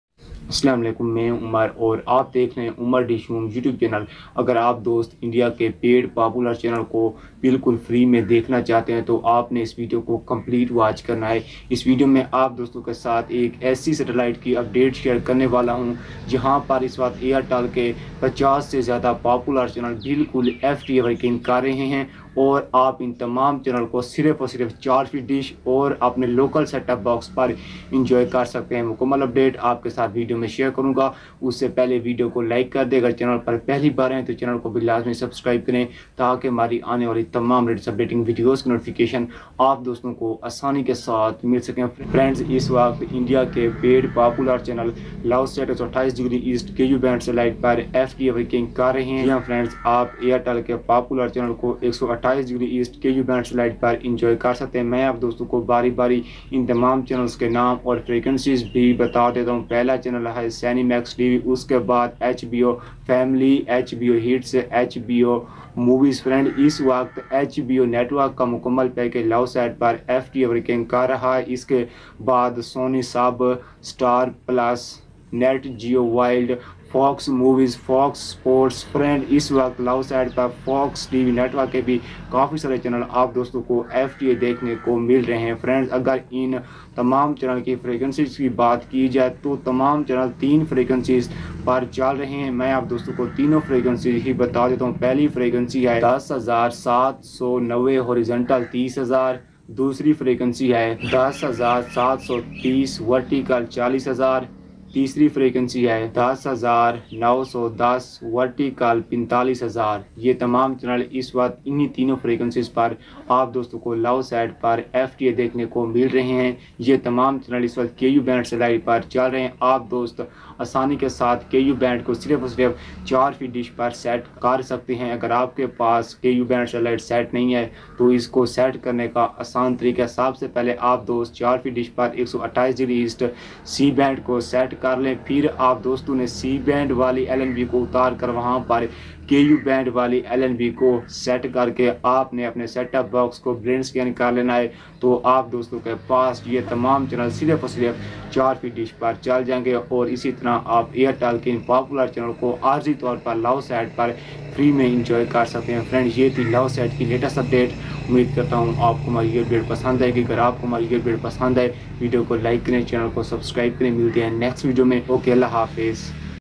Big good news for Indian popular channels added on Ku laosat 128E | HBO and fox tv network on 128E
Big good news for Indian popular channels added on Ku laosat 128E  | HBO and fox tv network on 128E  | Umar Dish Home

is video mein apke sath laosat 128e ku band satellite ki new update share krne wala hoon kha laosat per India ke popular channels add ho gaye hain. complete information ke liye video ko complete watch kren.